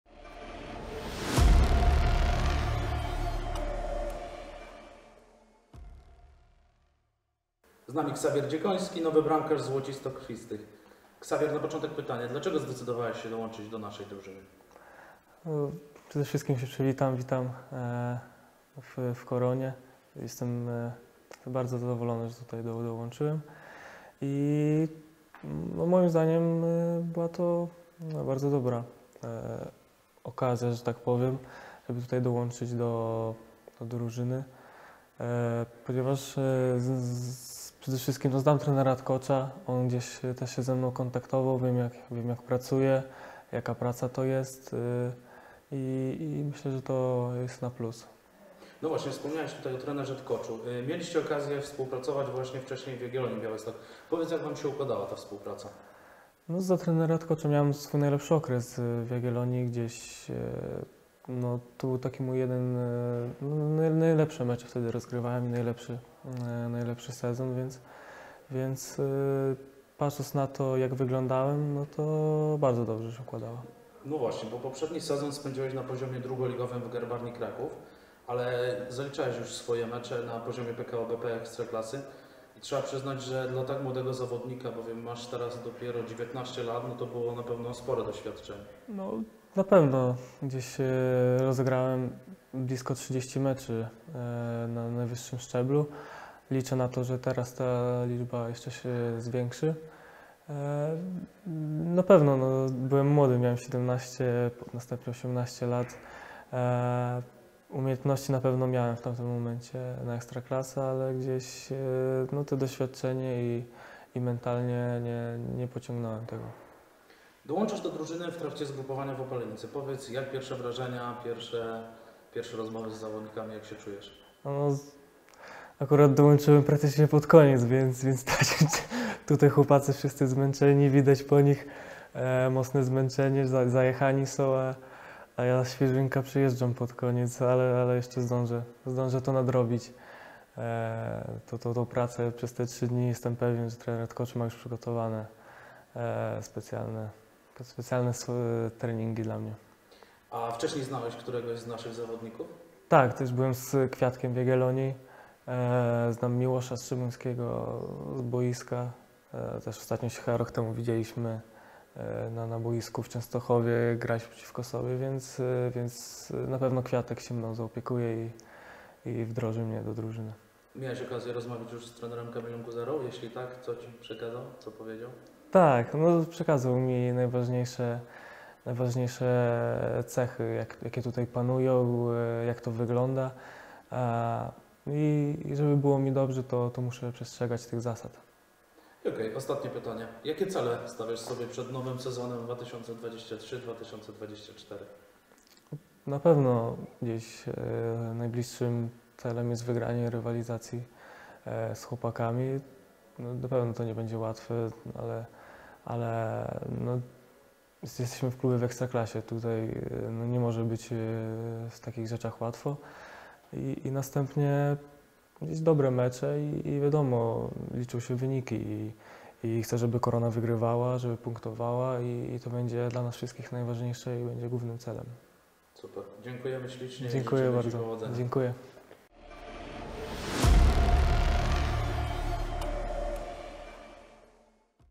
Xavier, witamy w Koronie!
- Cieszę się, że dołączam do Korony. Jako bramkarz miałem okazję współpracować wcześniej z trenerem Jarosławem Tkoczem, wiem jak pracuje i myślę, że dzięki tej pracy mogę tylko zyskać - mówi nowy zawodnik Złocisto-Krwistych, Xavier Dziekoński.